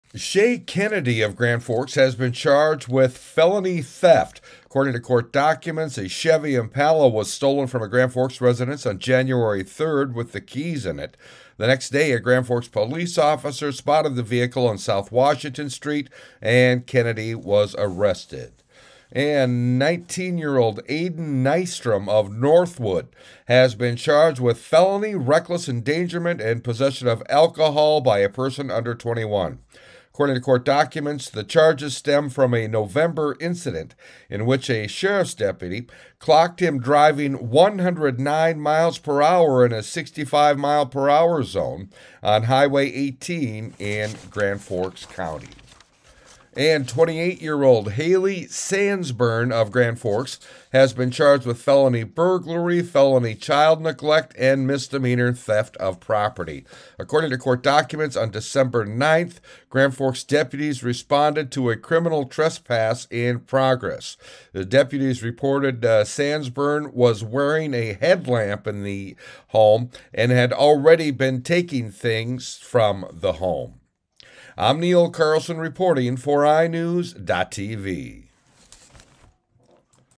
Recent Felony Level Charges
GRAND FORKS, ND (iNewZ.TV) A review of recent Felony level charges in Grand Forks District Court.